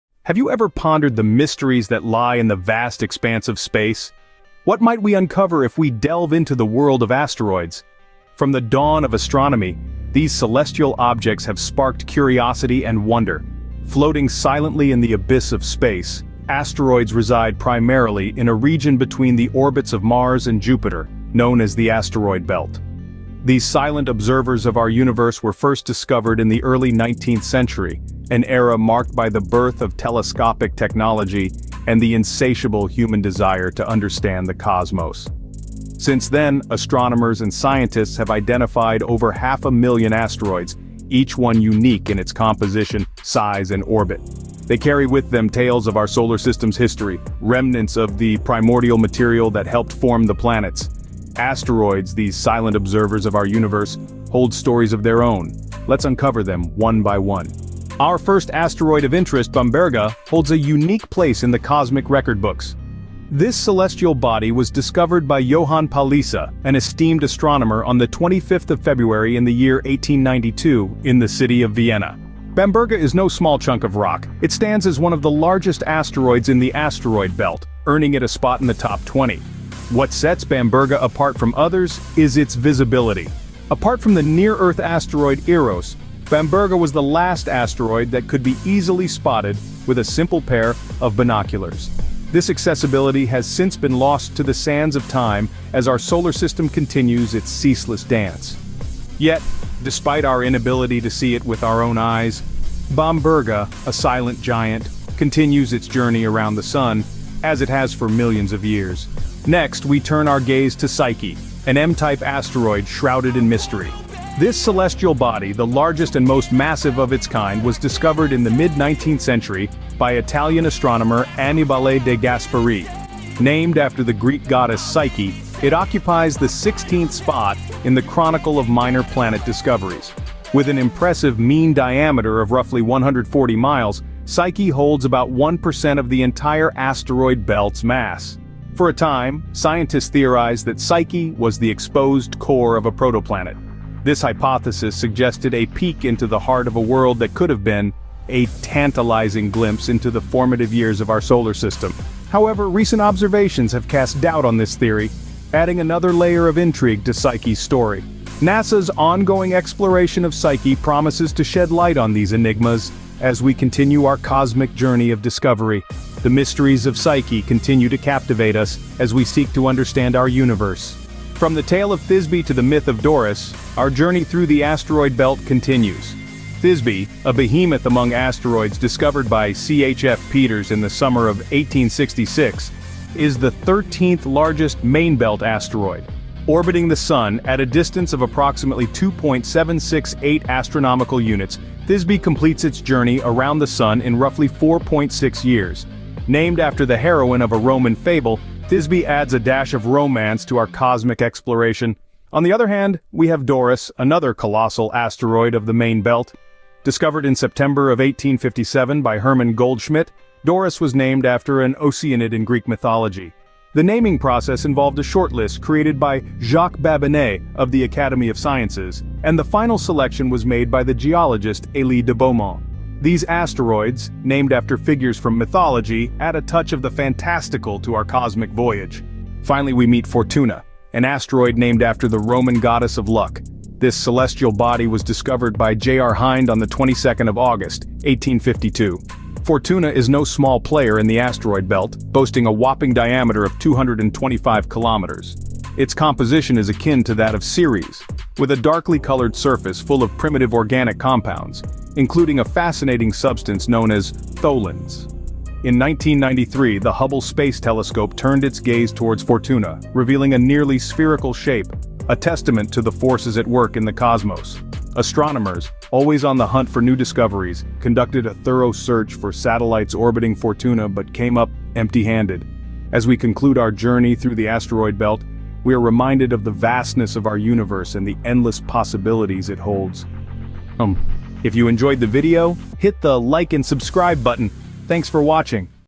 The Giants of the Asteroid Belt: A Journey Through Space
OUTLINE:

00:00:00 The Unseen Giants of Space
00:00:57 The Discovery of Bamberga
00:01:49 The Mysteries of Psyche
00:02:51 The Tale of Thisbe and Doris
00:03:49 The Fortune of Fortuna
00:04:41 Untitled Chapter